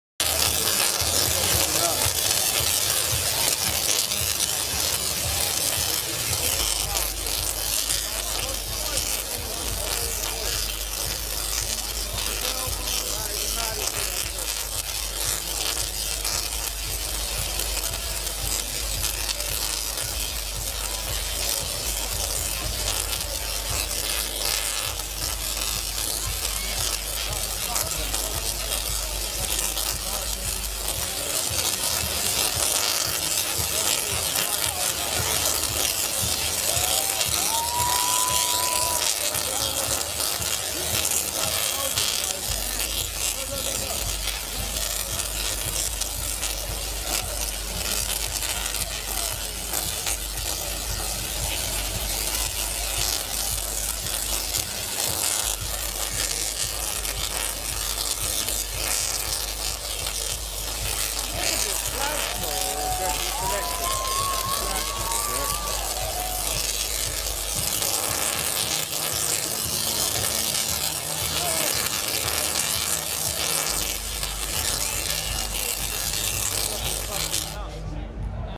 Lightning fighters At Electric Picnic
‎2 Guys having like a duel from gladiators down at electric picnic amazin